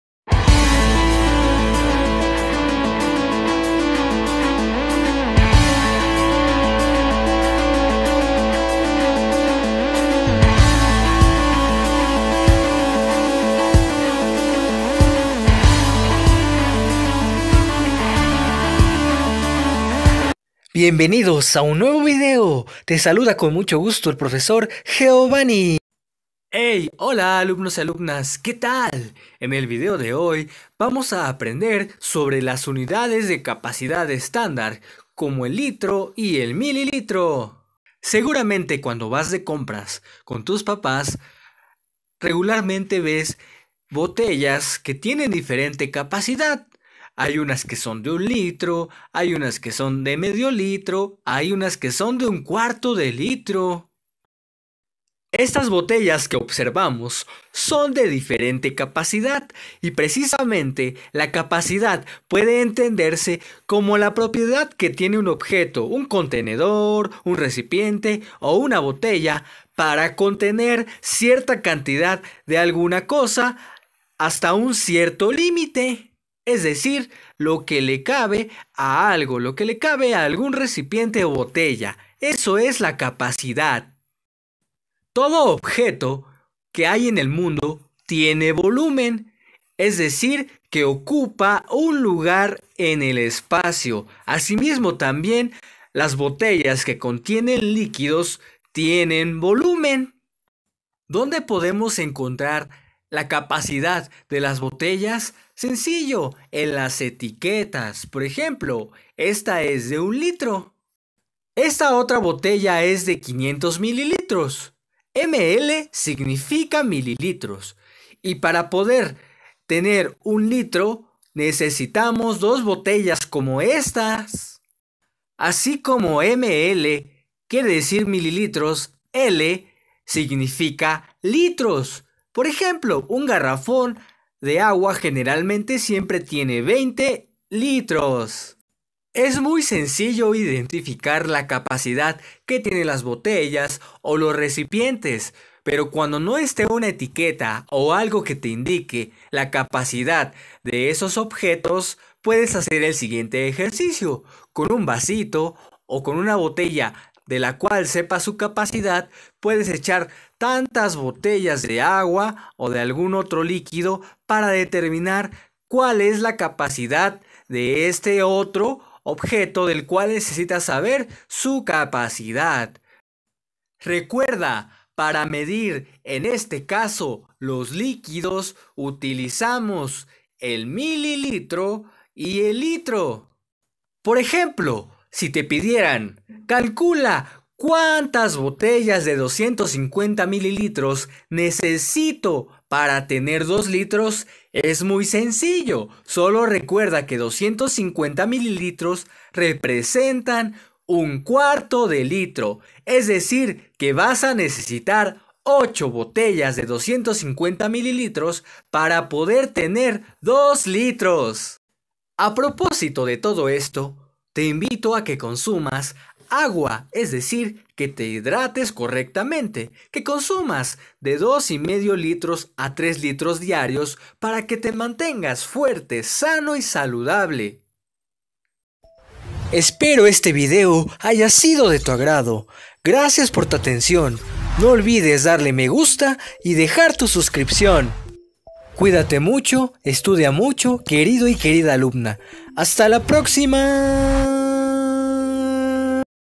Matemáticas Aprende en Casa II/ Unidades de capacidad litros militros.
En el video analizamos las unidades de medida para los líquidos, litros, militros.

Te invito a que me sigas en mi página de Facebook: Profesor Geovanni